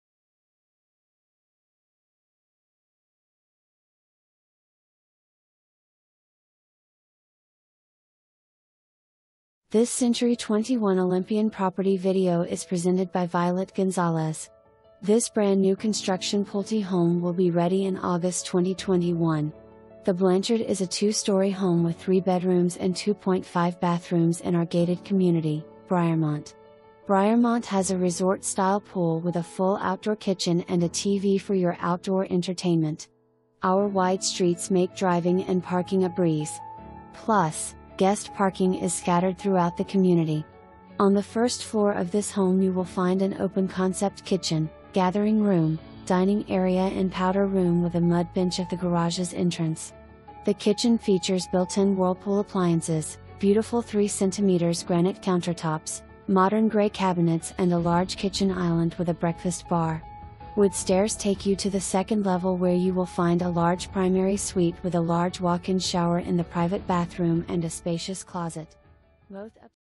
Century 21 Olympian - 3104 Dakota Falls Drive
Please enjoy this property video created by Century 21 Olympian

This brand new construction Pulte Home will be ready in August 2021. The Blanchard is a 2-story home with 3 Bedrooms and 2.5 Bathrooms in our gated community, Briarmont. Briarmont has a resort-style pool with a full outdoor kitchen and a TV for your OUTDOOR entertainment. Our wide streets make driving and parking a breeze! Plus, guest parking is scattered throughout the community. On the first floor of this home you will find an open concept Kitchen, Gathering Room, Dining Area and Powder Room with a Mud Bench at the Garage's entrance. The Kitchen features built-in Whirlpool appliances, beautiful 3 cm granite countertops, modern gray cabinets and a large Kitchen Island with a Breakfast Bar. Wood stairs take you to the second level where you will find a large Primary Suite with a large walk-in shower in the private bathroom and a spacious closet. Both upstairs Bathrooms have dual sinks.

2 full bath, 1 half bath

Violet Gonzalez
Century 21 Olympian